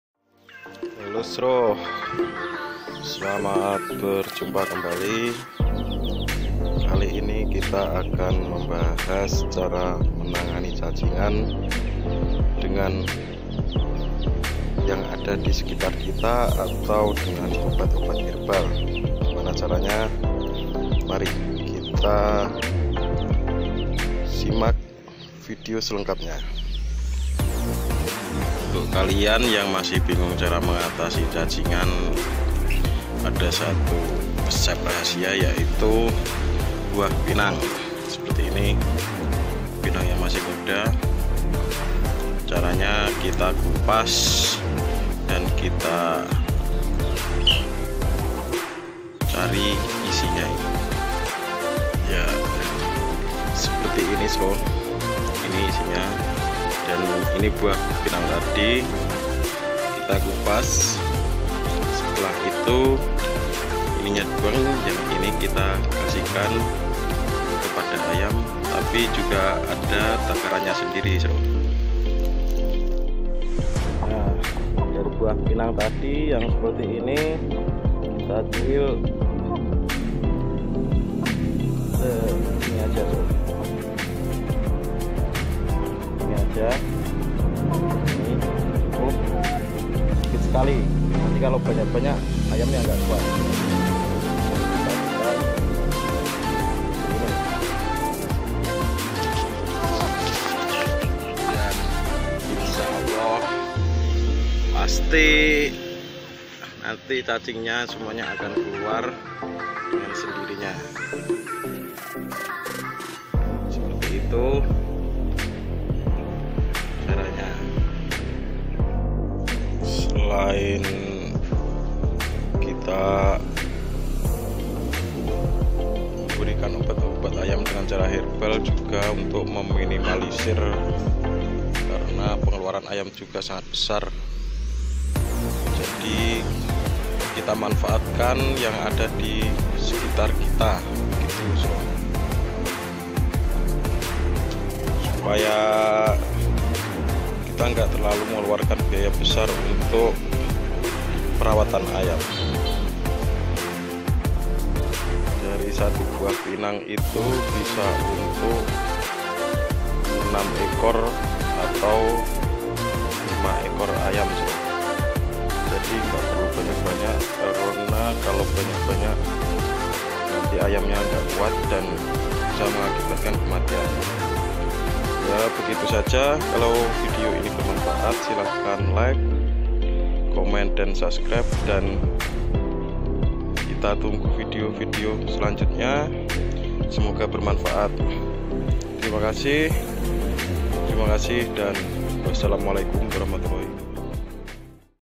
mengobati ayam cacingan dengan buah jambe!!
cara alami mengatasi ayam cacingan dengan buah jambe.
buah jambu terbukti ampuh mengobati ayam cacingan dengan cepat.

semoga bermanfaat.